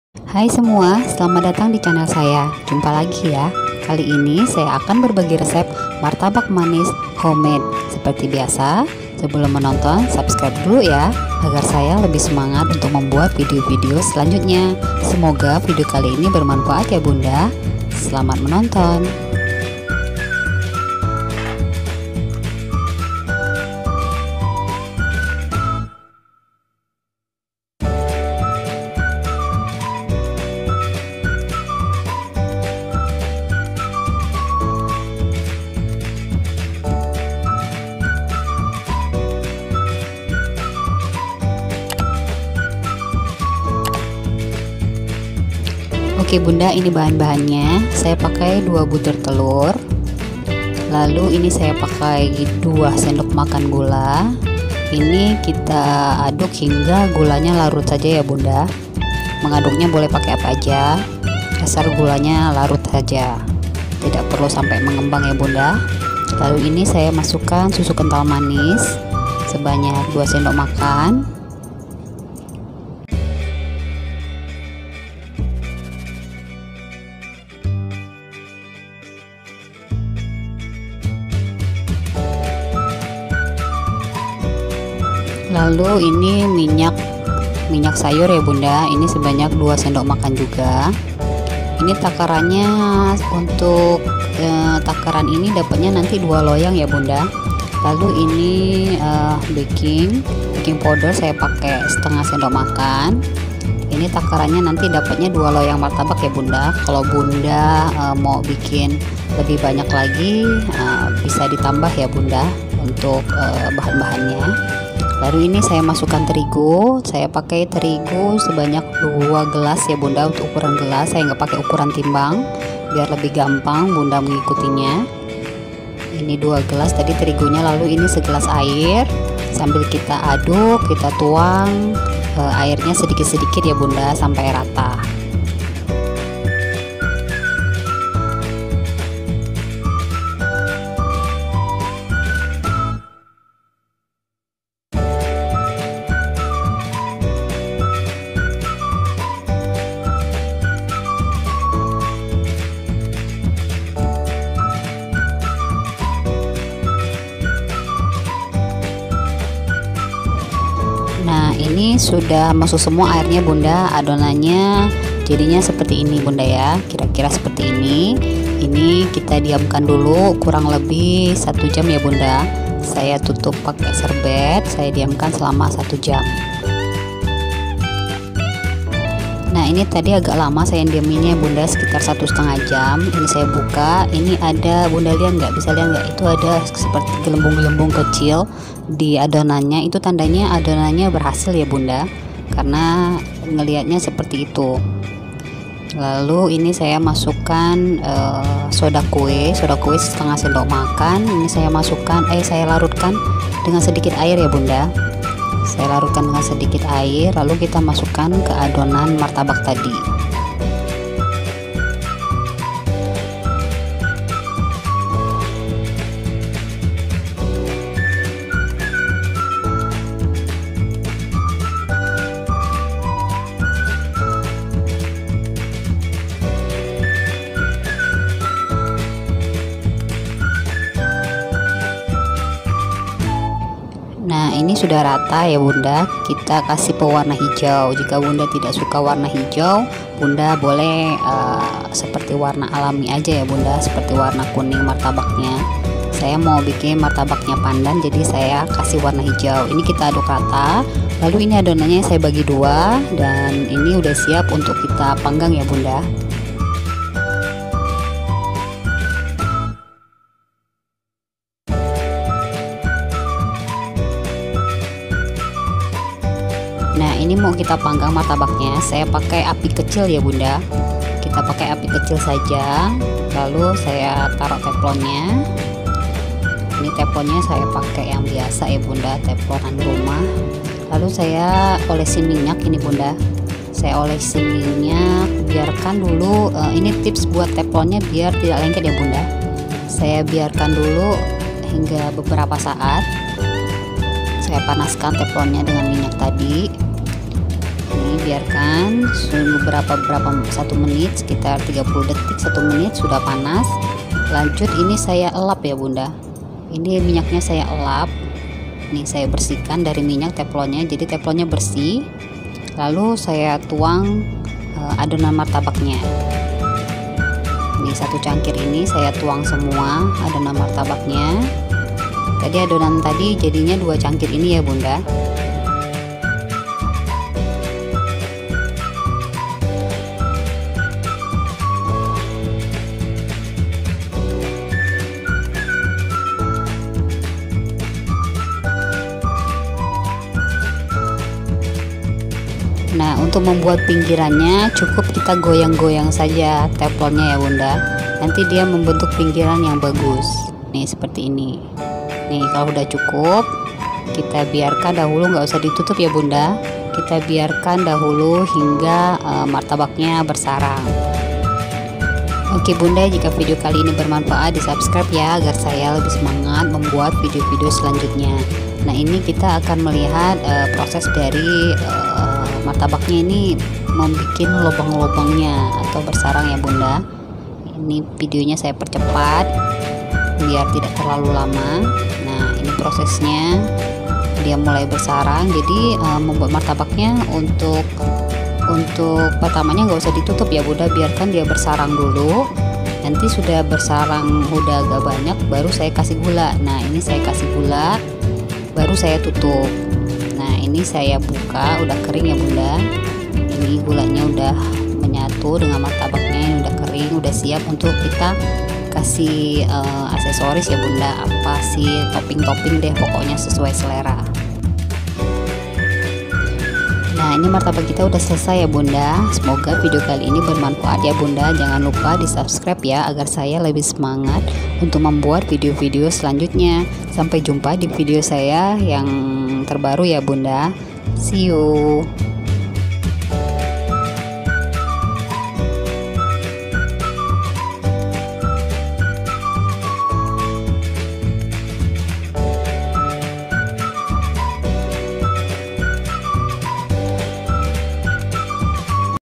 Cara memasak martabak manis ..homemade..tips teflon tdk lengket..
Haai..kali ini saya akan berbagi cara martabak manis...homemade..ada byk tips bunda..selamat mencoba bunda...smoga bermanfaat ya..

Bahan bahan :
susu 2 sdm
minyak 2 sdm
baking powder 1/2 sdm
terigu 2 gelas
air 1 gelas
soda kue 1/2 sdm
pasta pandan

Apabila ada saran dan pertanyaan atau teman-teman ingin request silakan komen di bawah..

Terimakasih..😍🥰

Tonton juga :

*Masak bolu pandan lembut..dijamin
*Nastar ambyar dimulut..bisa isi coklat jg..

Edit tumbnail : PicsArt